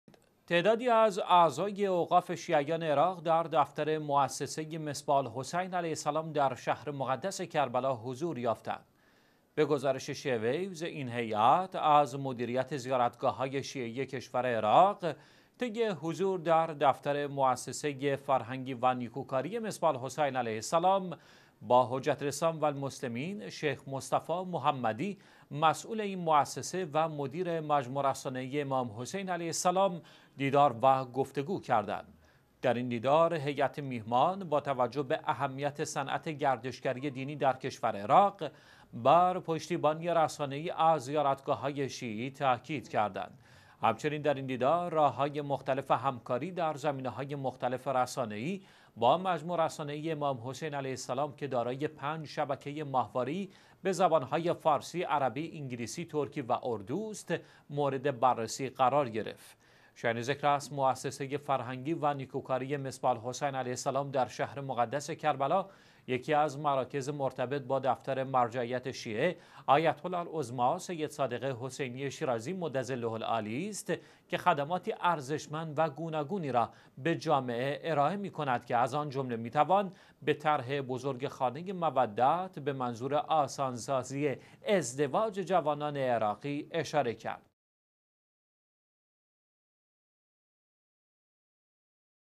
دیدار هیئتی از اوقاف شیعیان عراق با مدیر موسسه مصباح الحسین علیه السلام
تعدادی از اعضای اوقاف شیعیان عراق، در دفتر موسسه «مصباح الحسین علیه السلام» در شهر مقدس کربلا حضور یافتند.
به گزارش شیعه ویوز، این هیئت از مدیریت زیارتگاه های شیعی کشور عراق، طی حضور در دفتر موسسه فرهنگی و نیکوکاری مصباح الحسین علیه السلام، با حجت الاسلام و المسلمین «شیخ مصطفی محمدی» مسئول این موسسه و مدیر مجموعه رسانه‌ای امام حسین علیه السلام، دیدار و گفت و گو کردند.
در این دیدار، هیئت میهمان با توجه به اهمیت صنعت گردشگری دینی در کشور عراق، بر پشتیبانی رسانه‌ای از زیارتگاه‌های شیعی تاکید کردند.
همچنین در این دیدار راه‌های مختلف همکاری در زمینه‌های مختلف رسانه‌ای با مجموعه رسانه‌ای امام حسین علیه السلام که دارای 5 شبکه ماهواره ای به زبان های فارسی، عربی، انگلیسی، ترکی و اردو است، مورد بررسی قرار گرفت.
 شایان ذکر است، موسسه فرهنگی و نیکوکاری مصباح الحسین علیه السلام در شهر مقدس کربلا، یکی از مراکز مرتبط با دفتر مرجعیت شیعه آیت الله العظمی سید صادق حسینی شیرازی مدظله العالی است که خدماتی ارزشمند و گوناگونی را به جامعه ارائه می کند، که از آن جمله می‌توان به طرح بزرگ «خانه مودت» به منظور آسان سازی ازدواج جوانان عراقی، اشاره کرد.